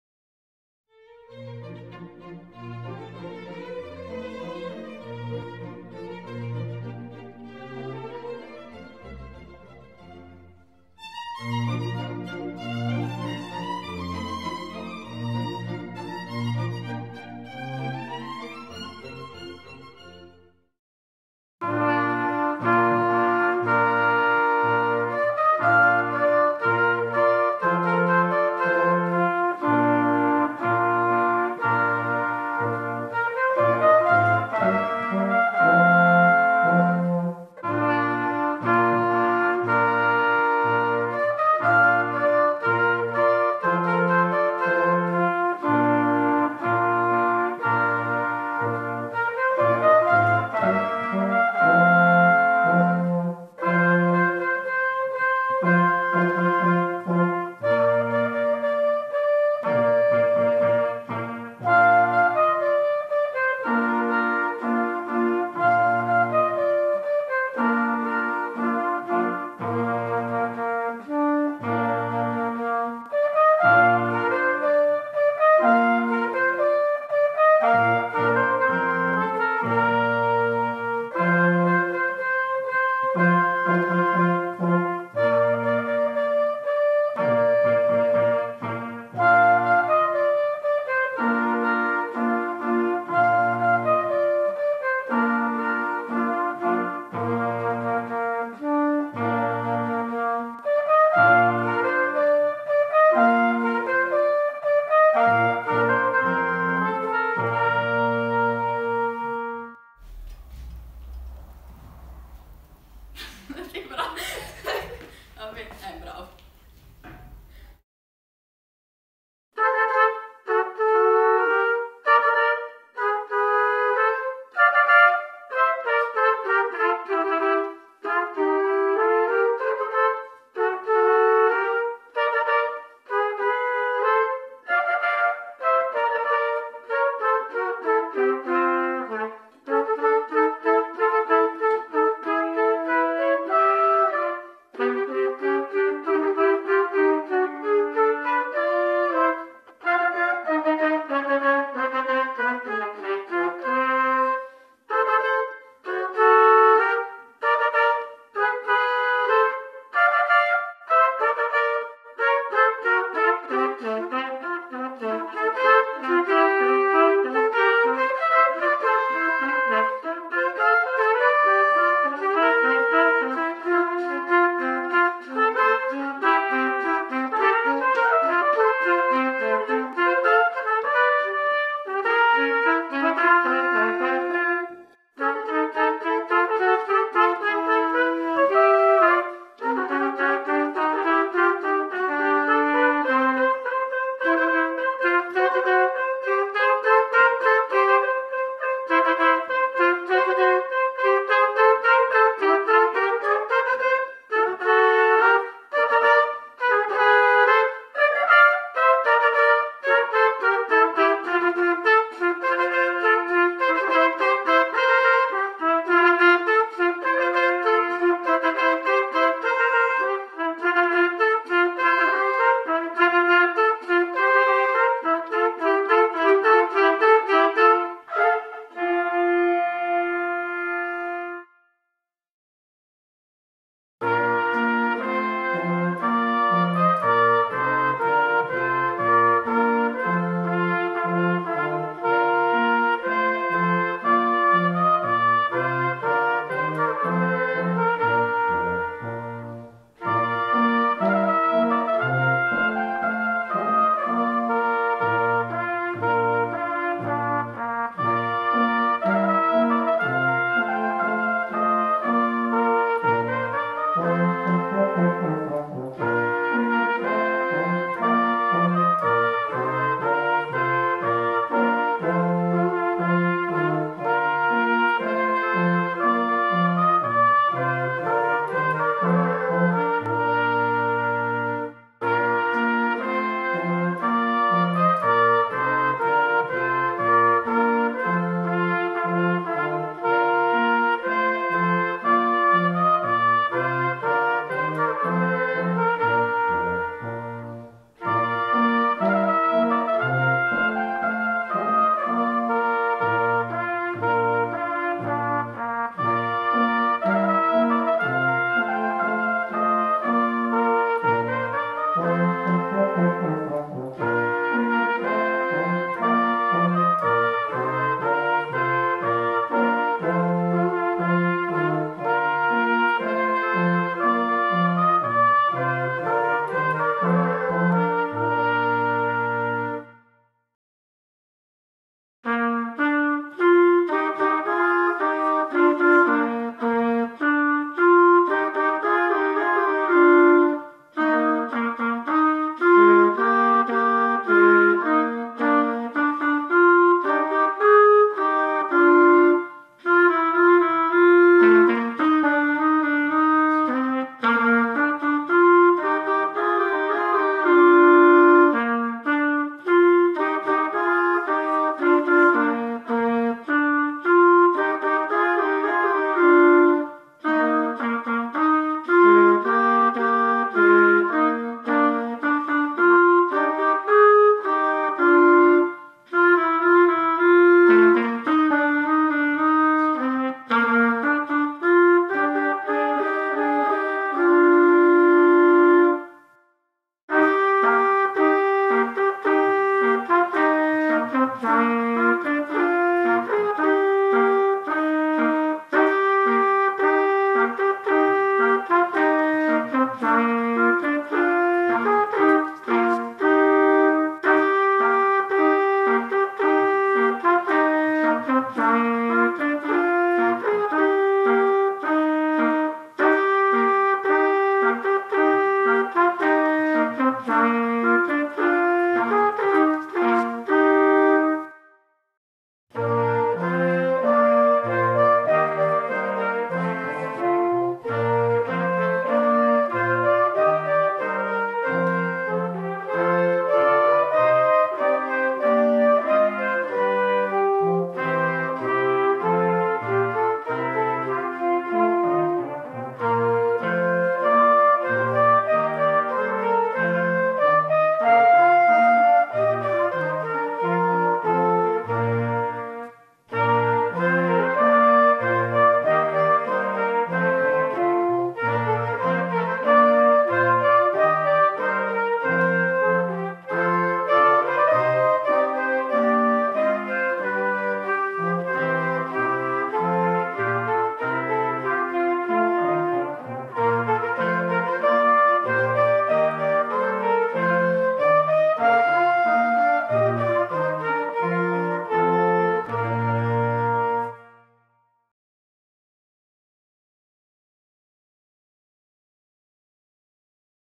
Saggi 2021 - Classe di ottoni
Saggio della classe di ottoni della Filarmonica G. Rossini